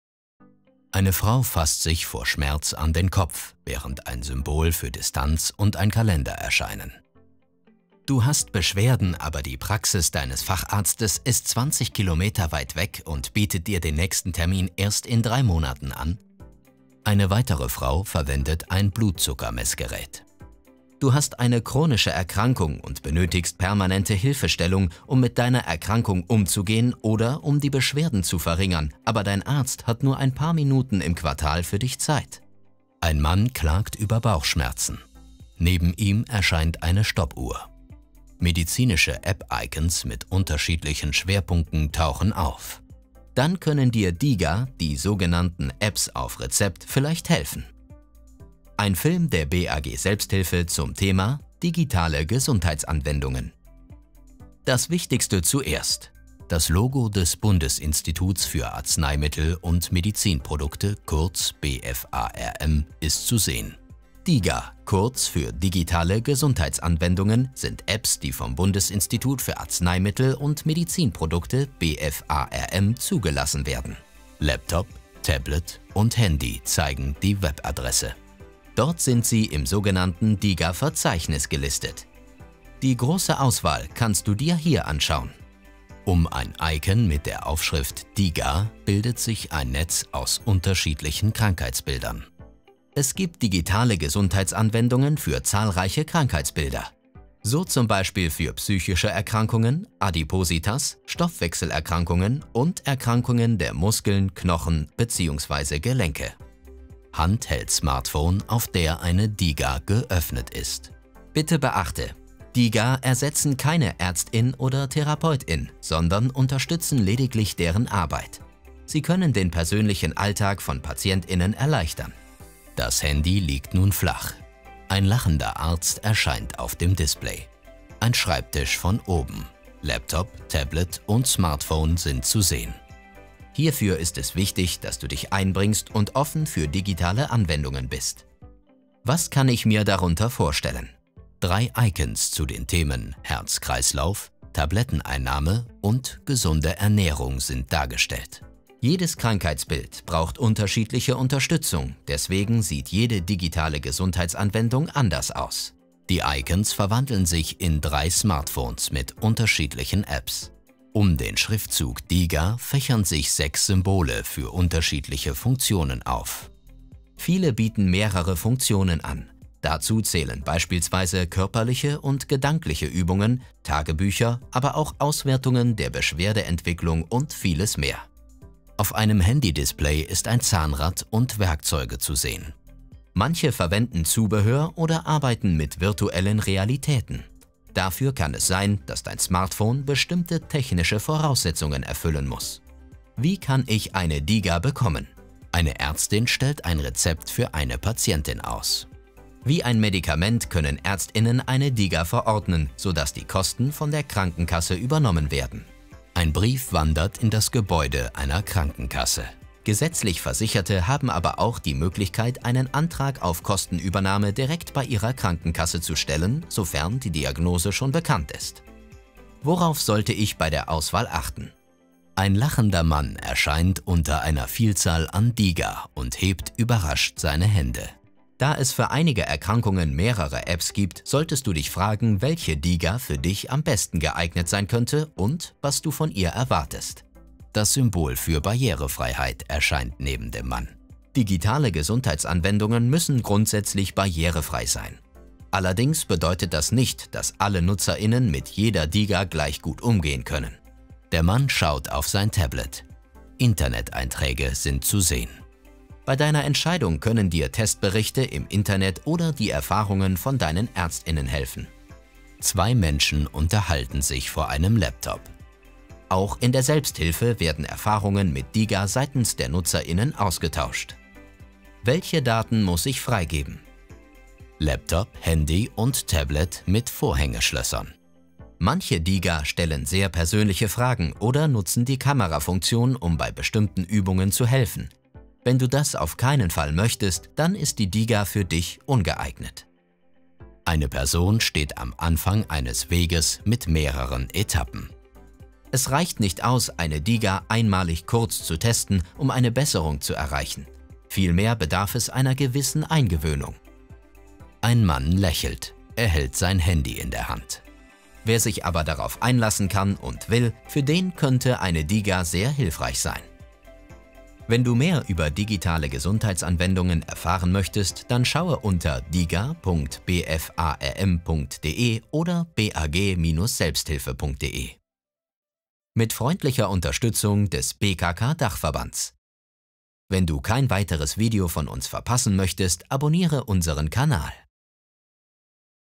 DiGA: Digitale Gesundheitsanwendungen auf Rezept (mit Audiodeskription)
Seit dem Jahr 2020 gibt es eine neue Möglichkeit für Menschen die mit chronischen Erkrankungen leben: Die "App auf Rezept". Aber was verbirgt sich hinter diesem Konzept?

DiGA, kurz für digitale Gesundheitsanwendungen, sind speziell entwickelte Apps, die von staatlicher Seite genehmigt wurden. Diese Apps können von Ärzten verschrieben werden und haben das Potenzial, das Leben junger Menschen mit chronischen Erkrankungen erheblich zu verbessern.

DiGA bieten eine breite Palette von Lösungen für verschiedene gesundheitliche Herausforderungen, insbesondere für chronische Erkrankungen. Dazu gehören Krankheitsbilder wie psychische Erkrankungen, Adipositas, Stoffwechselerkrankungen sowie Erkrankungen des muskuloskelettalen Systems.

Wichtig ist zu betonen, dass DiGA keine ärztlichen Konsultationen ersetzen, aber sie können eine wertvolle Ergänzung sein, um eure Gesundheit und Lebensqualität zu fördern.

Und hier ist die gute Nachricht: Unser informativer Film ist speziell auf euch zugeschnitten und verfügt über eine Audiodeskription, um sicherzustellen, dass die Informationen für Alle zugänglich sind. Schaut euch den Film an, um nützliche Tipps zur Nutzung von DiGA zu erhalten.

Besucht auch unser offizielles DiGA-Verzeichnis, um eine Übersicht über die verfügbaren Anwendungen zu erhalten